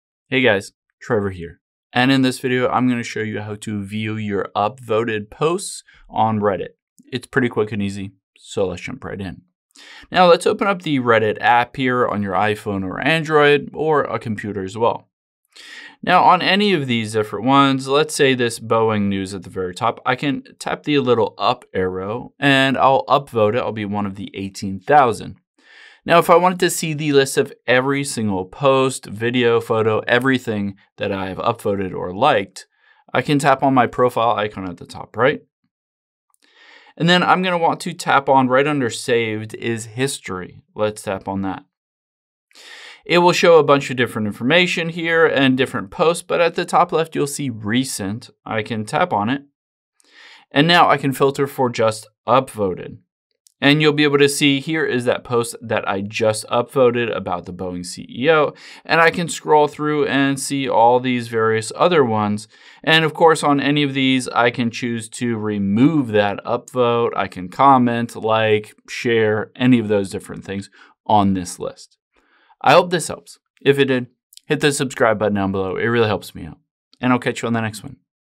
How To View Upvoted Posts On Reddit
Here's how to see every post that you've upvoted on Reddit if you want to go back and read them again or comment on them.